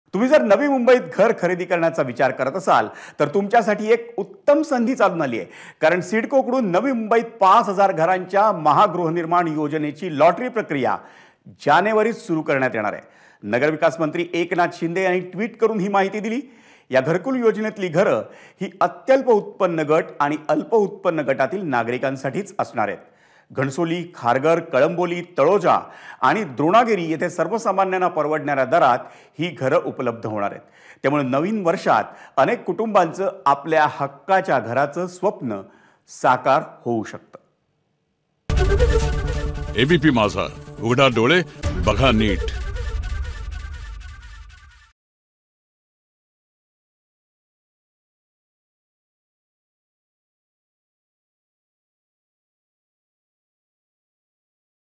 CIDCO : जानेवारीत सिडकोकडून नवी मुंबईत पाच हजार घरांसाठी लॉटरी, मंत्री एकनाथ शिंदेंची माहिती
तुम्ही नवी मुंबईत घर खरेदी करण्याचा विचार करत असाल तर तुमच्यासाठी एक उत्तम संधी चालून आली आहे. कारण सिडकोकडून नवी मुंबईत पाच हजार घरांच्या ‘महागृहनिर्माण’ योजनेची लॉटरी प्रक्रिया जानेवारीत सुरू करण्यात येणार आहे. नगरविकास मंत्री एकनाथ शिंदे यांनी ट्विट करून ही माहिती दिली आहे. या घरकुल योजनेतली घरं अत्यल्प उत्पन्न गट, आणि अल्प उत्पन्न गटांतील नागरिकांसाठी असणार आहेत. घणसोली, खारघर, कळंबोली, तळोजा आणि द्रोणागिरी येथे सर्वसामान्यांना परवडणाऱ्या दरात ही घरं उपलब्ध होणार आहेत. त्यामुळं नवीन वर्षात अनेक कुटुंबांचं आपल्या हक्काच्या घराचं स्वप्न साकार होणार आहे.